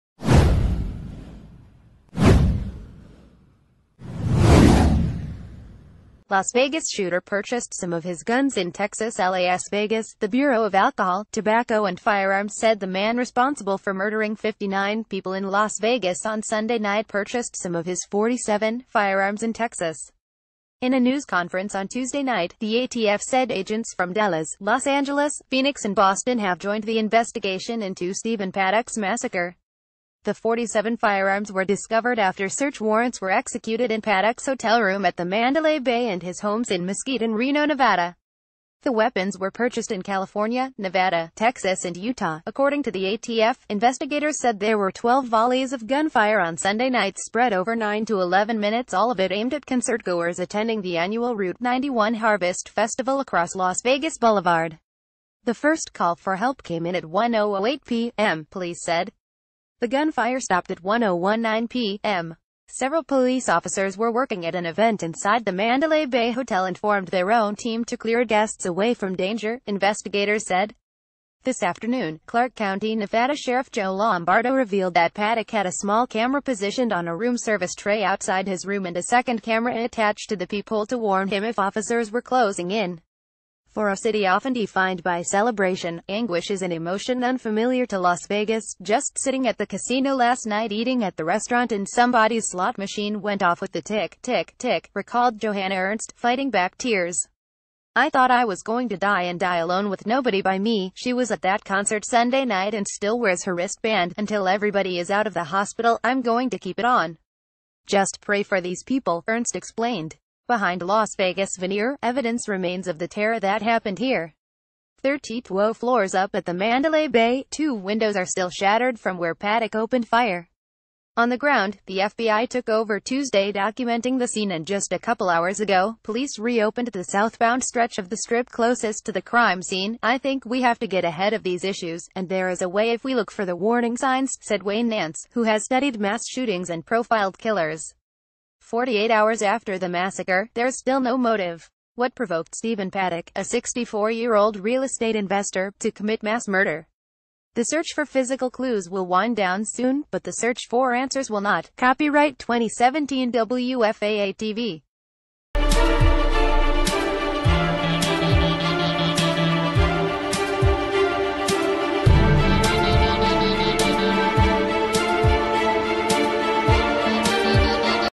Breaking News | Atf: las vegas shooter purchased some of his guns in texas
Atf: las vegas shooter purchased some of his guns in texas

Las Vegas shooter purchased some of his guns in Texas LAS VEGAS – The Bureau of Alcohol, Tobacco and...